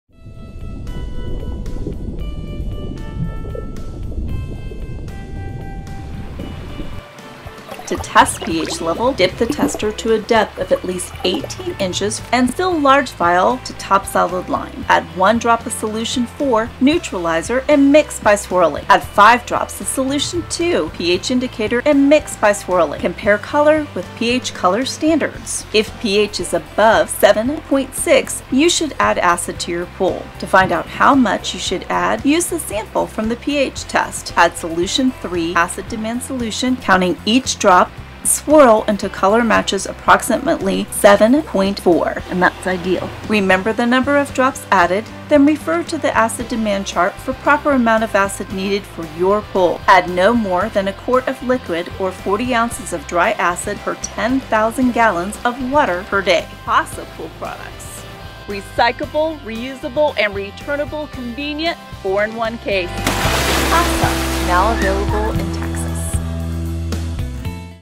How To Test Your Pool 4-in-1 Test Kit Ph and Acid Demand Test
4-in-1 Test Kit Testing your pool's Ph levels and Acid Demand.